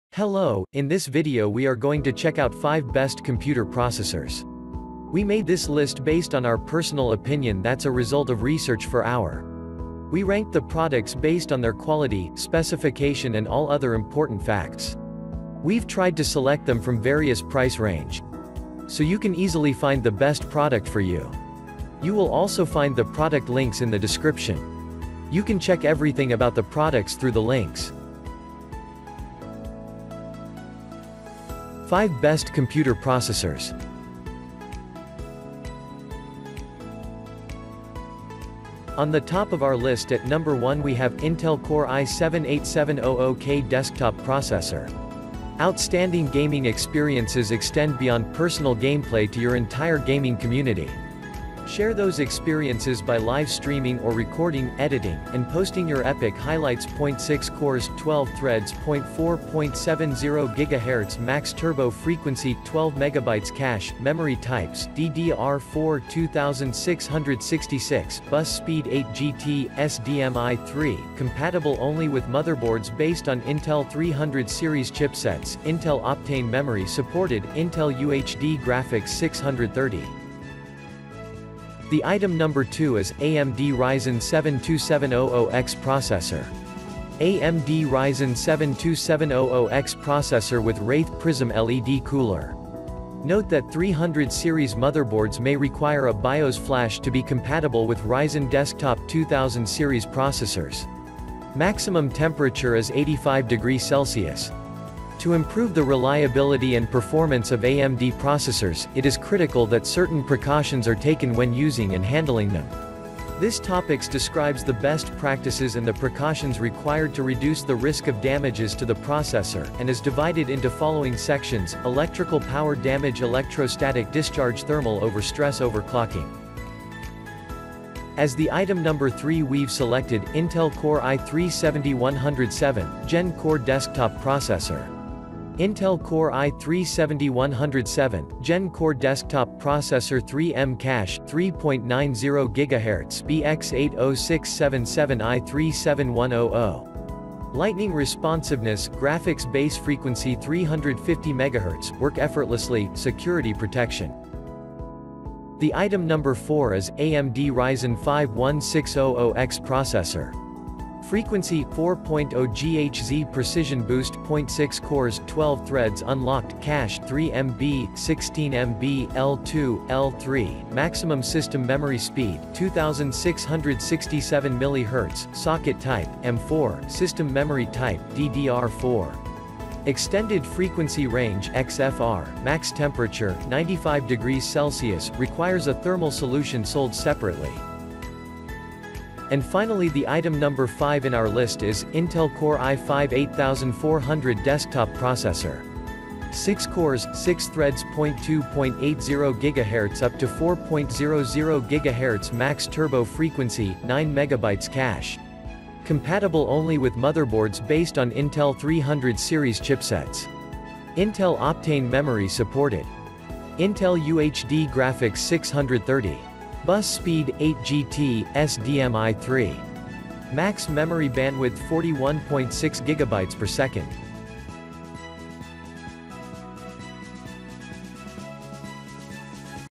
Top 5 Best Computer Processors 2020
► Links to the Computer Processors we listed in this video:

►1.Intel Core i7-8700K
►2.AMD Ryzen 7 2700X Processor
►3.Intel Core i3-7100
►4.AMD Ryzen 5 1600X Processor
►5.Intel Core i5-8400

In this video, we listed the 5 Best Computer Processors available in the market for this year. We made this list based on my personal opinion and i tried to list them based on their quality, durability, price,  and more.